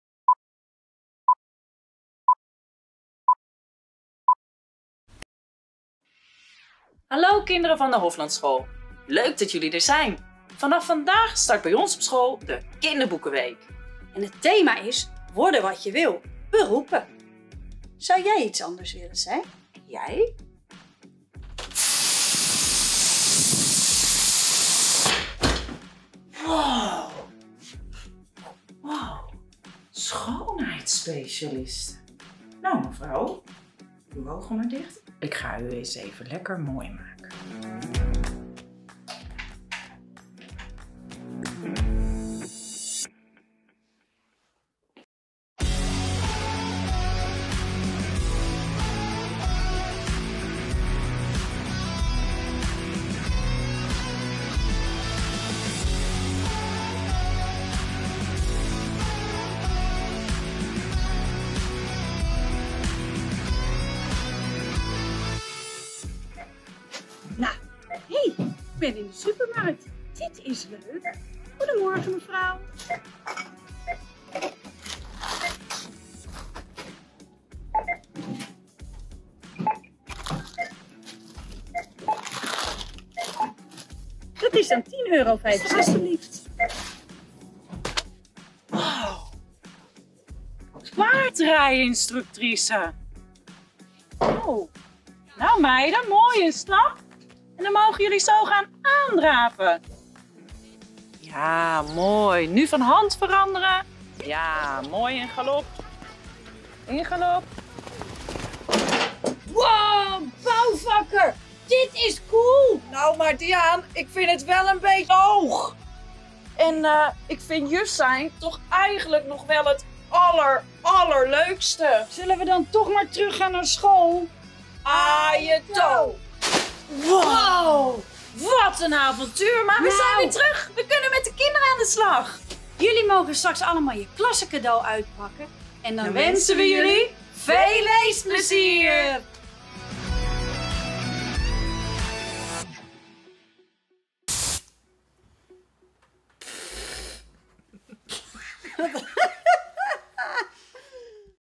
Hoflandschool Kinderboekenweek
Voor de Hoflandschool maakten wij samen met Juf Diana en Juf Patricia een nieuwe video voor de Kinderboekenweek 2021.
Camera/montage: Michel Rahn

Met dank aan:
Improvisatieacteurs
Albert Heijn Mijdrecht
Stal Brouwer Zadelpret
Schoonheidssalon Périne Mijdrecht
BasicFilm